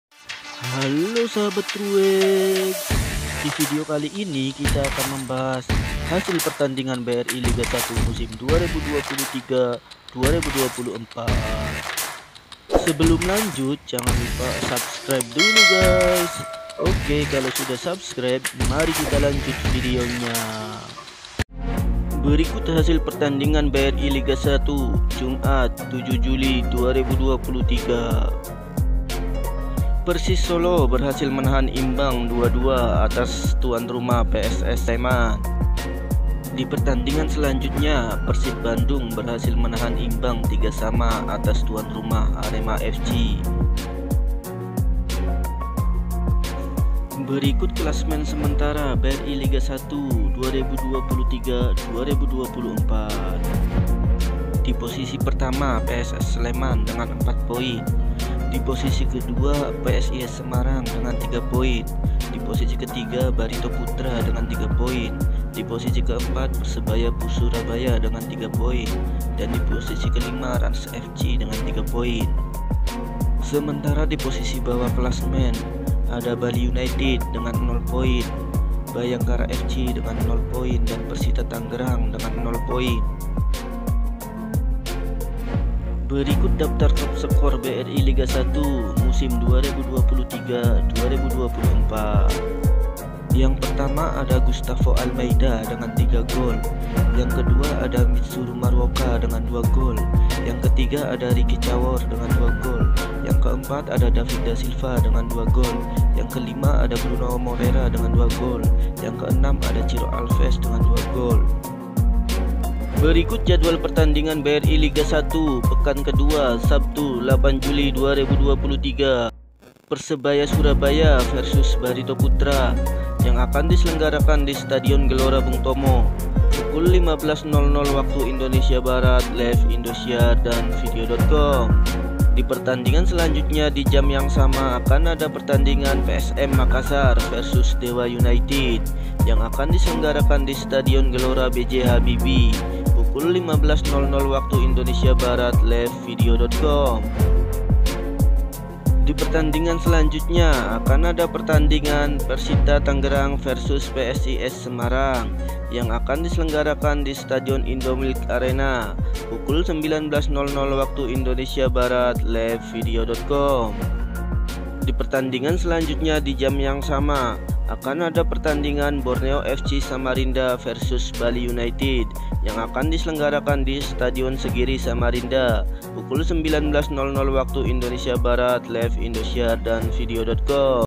Hasil BRI Liga 1 Hari ini ~ Arema fc vs Persib Bandung ~ Klasmen Terbaru BRI liga 1 2023/2024
Hasil pertandingan liga 1 hari ini

Pss Sleman 2-2 persis solo
Arema fc 3-3 persib bandung